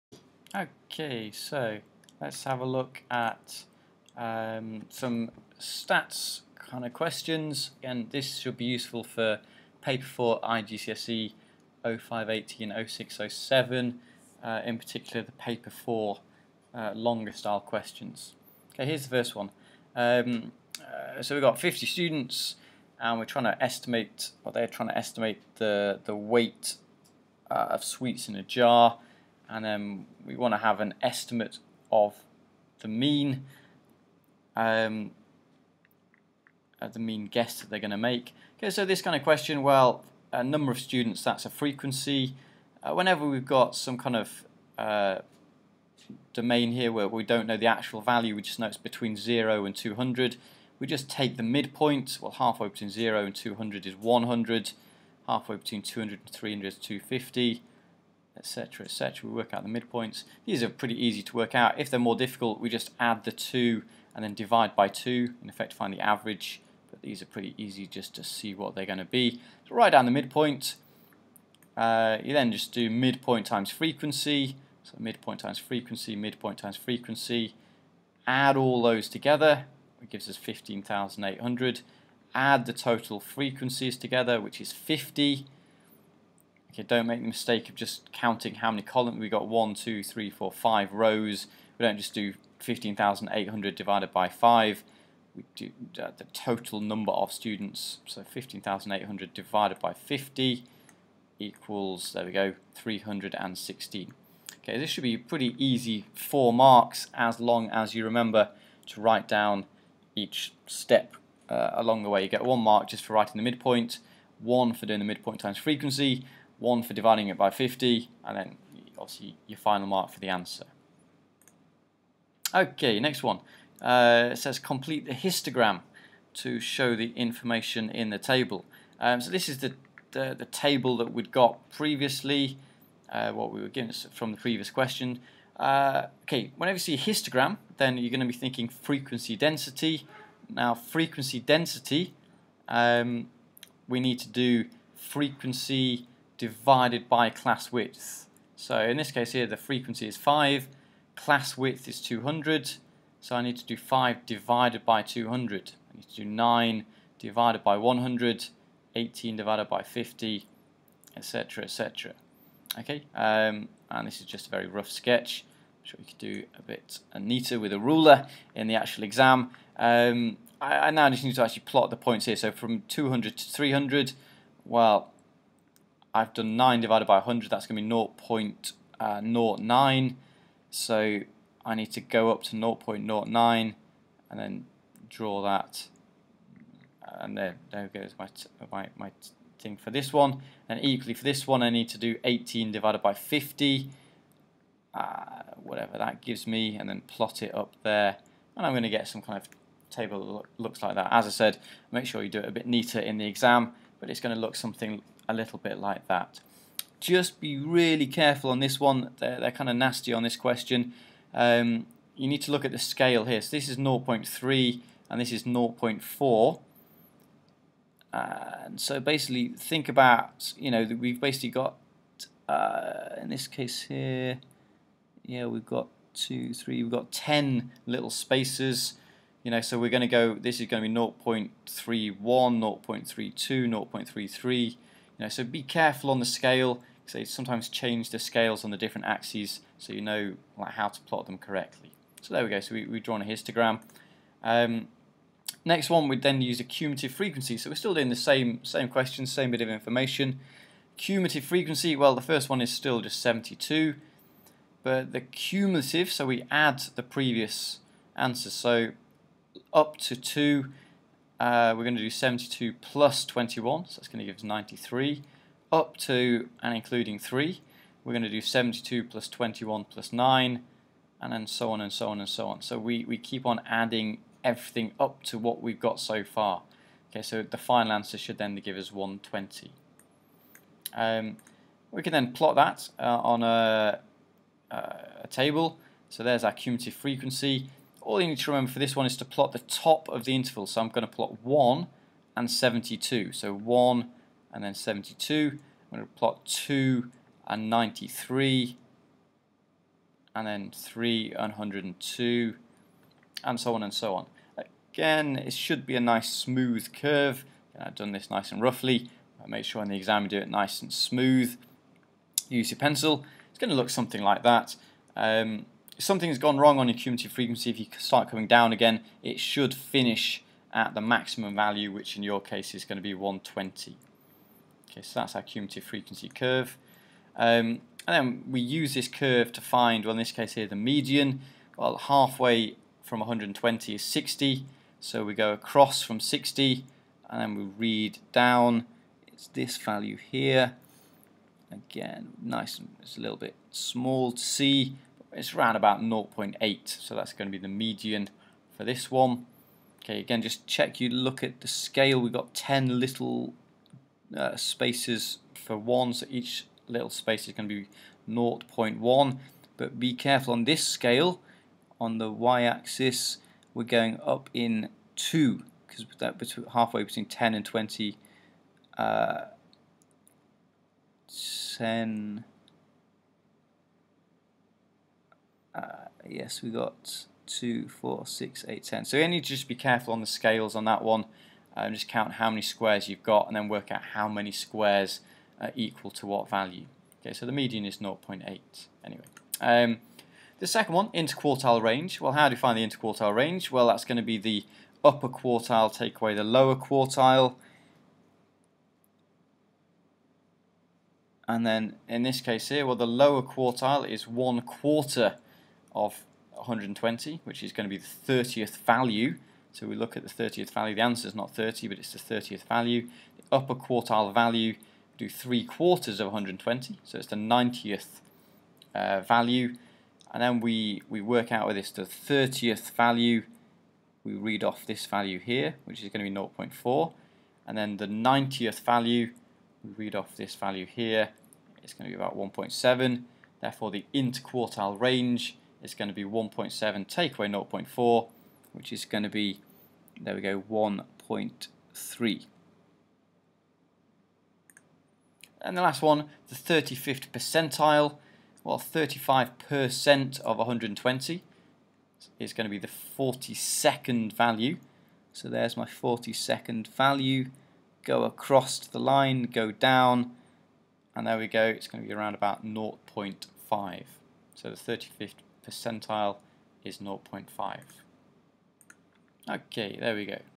Statistics: IGCSE Maths Extended Cambridge Past Paper Questions
IGCSE Mathematics exam questions (0580 and 0607) on statistics.   Including mean from a frequency table, cumulative frequency and histograms.